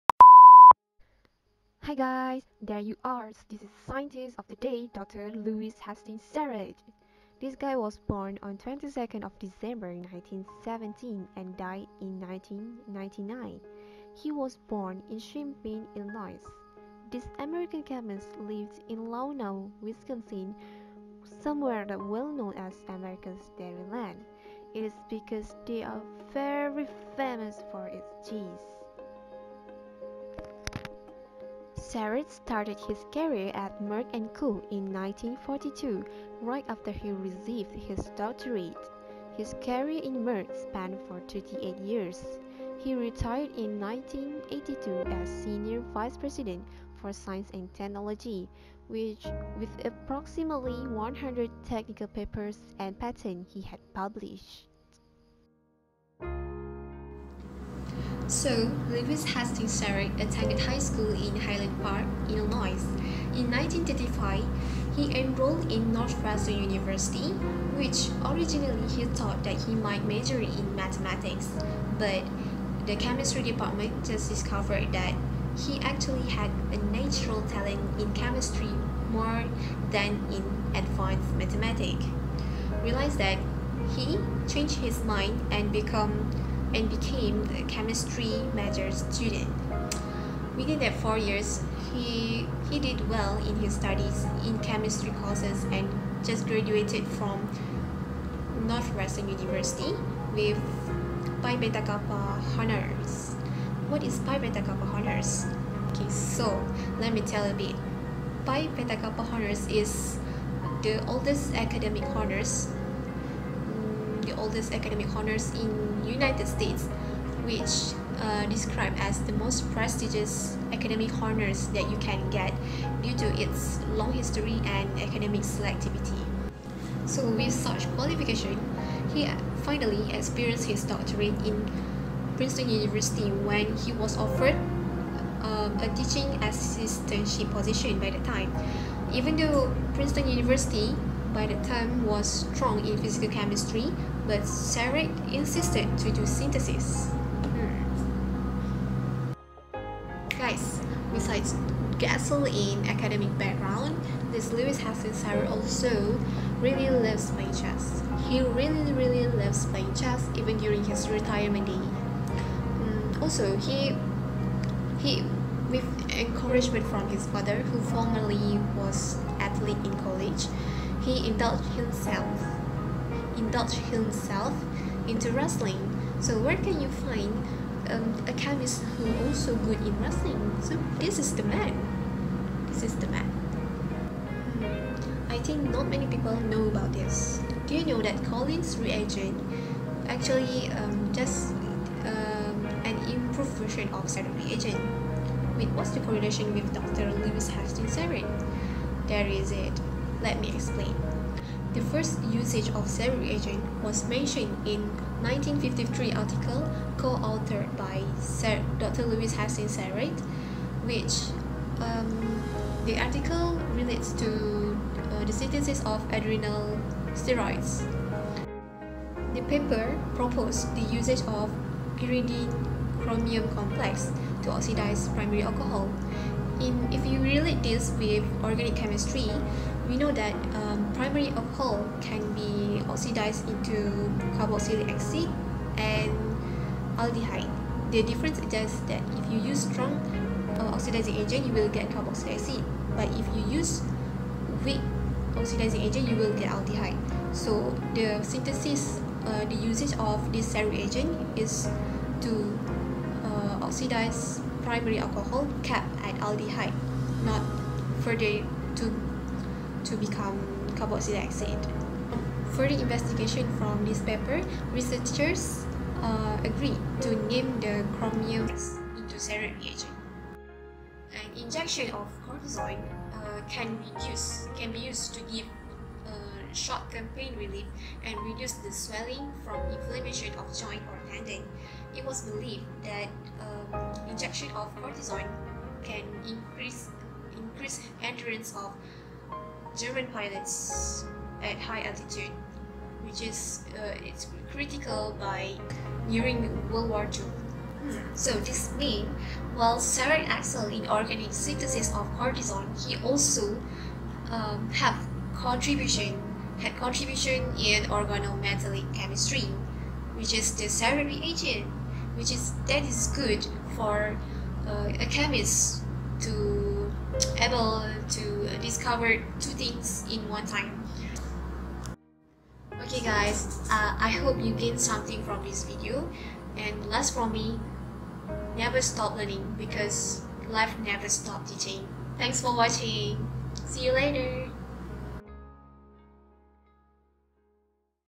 ORGANOMETALLIC CHEMIST : LEWIS HASTINGS SARETT
Lewis Hasting Sarett is an American organic chemist whom firstly prepared the synthetic cortisone in 1944. In the synthesis,  pyridine chromium complex was used to oxidize primary and secondary alcohol. Several years after, the complex was named after Lewis Hastings Sarett as Sarett reagent.